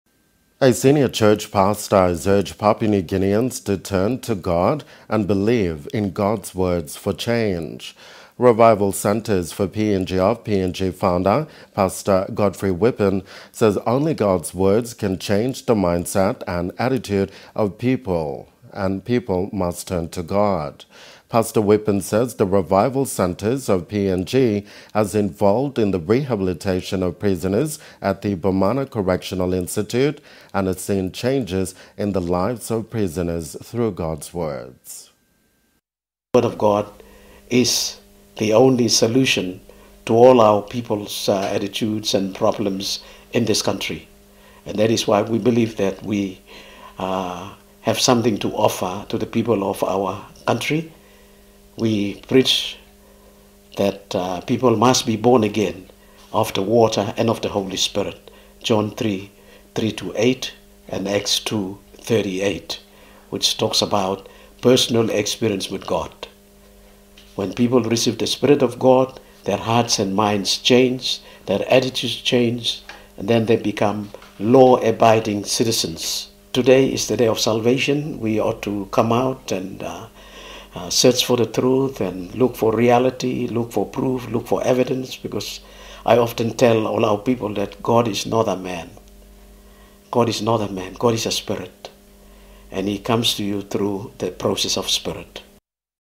God's Word Changes_NBC News
A senior pastor has urged Papua New Guineans to turn to God and believe in God's Word for change.